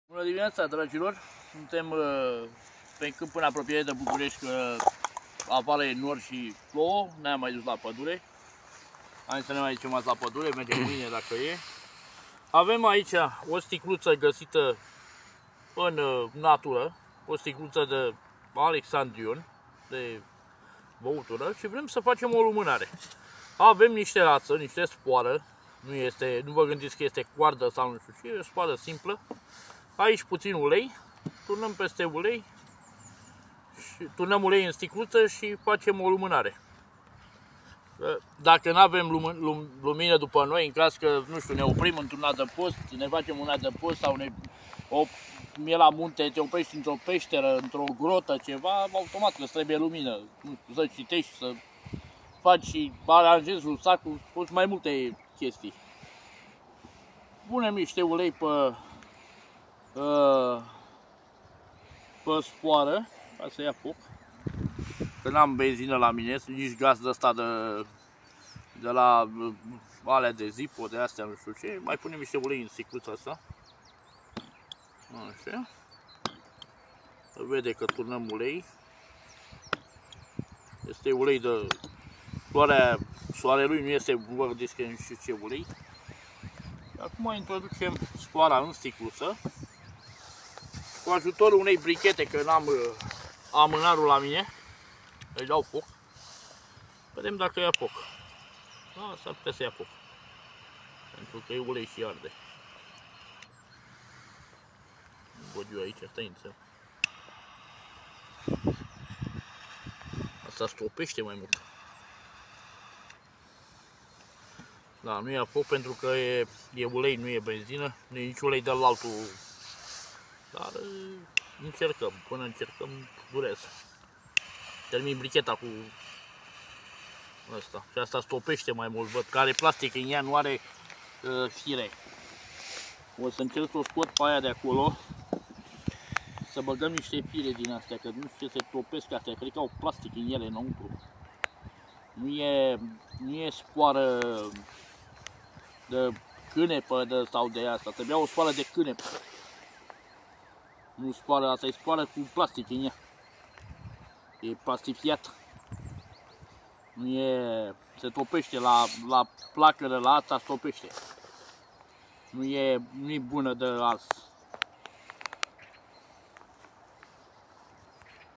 Lumanare facut in natura
Dintr-o sticluta gasita in natura ,putin ulei si o sfoara am creat acesta lumanare pentru a face lumina in adapost sau intr-o grota ( asta daca nu aveti altceva de aprins ( lanterna ) . )